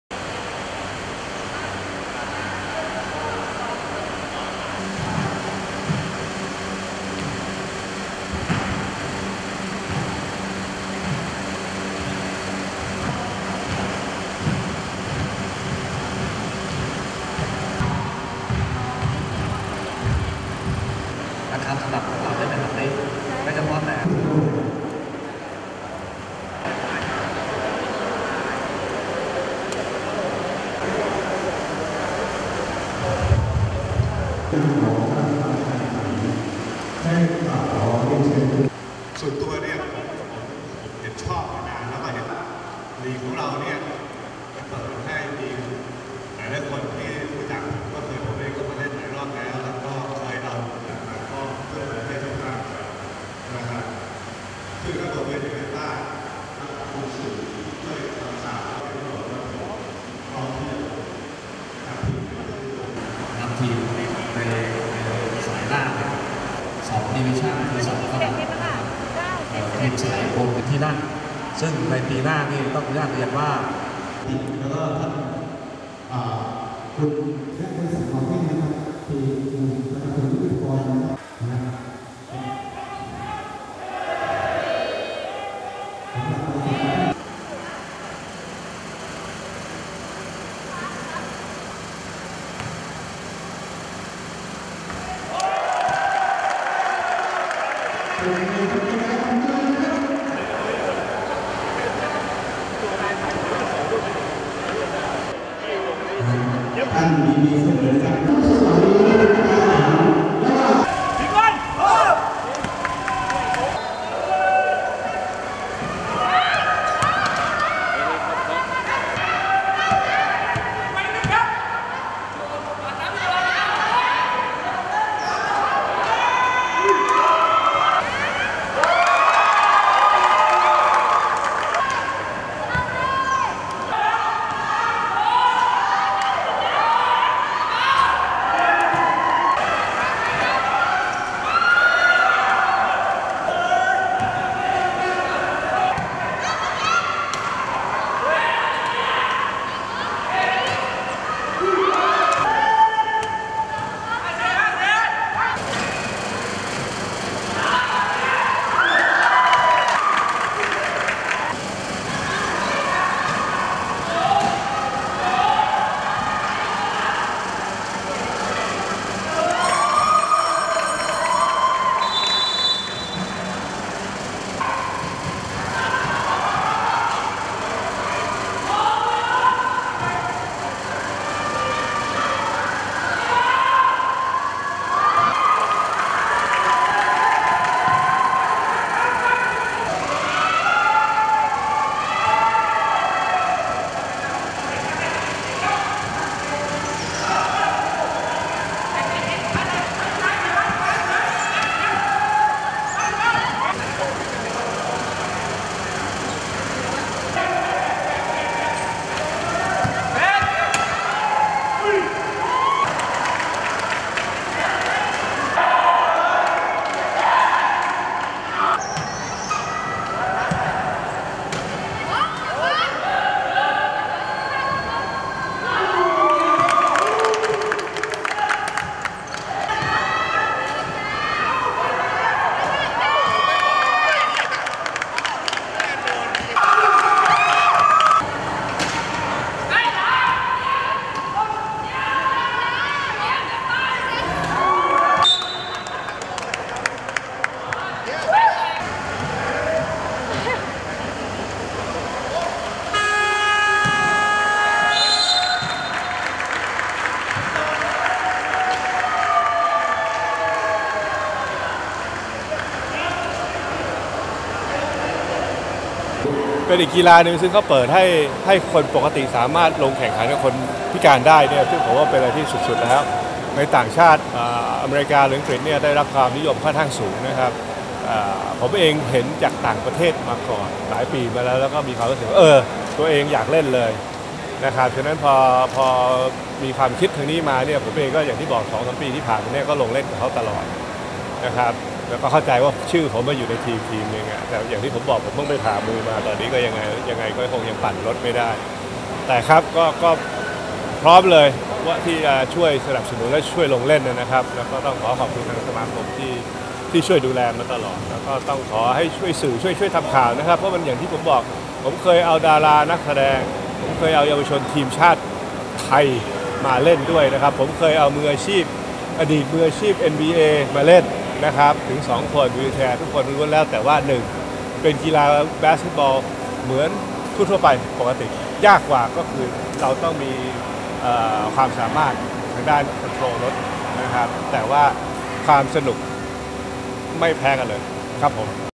ภาพข่าวพิธีเปิดการแข่งขันกีฬาวีลแชร์บาสเกตบอลชิงแชมป์ประเทศไทย ประจำปี 2564 วันที่ 27 กุมภาพันธ์ 64
นาย จุตินันท์ ภิรมย์ภักดี
ประธานคณะกรรมการพาราลิมปิกแห่งประเทศไทย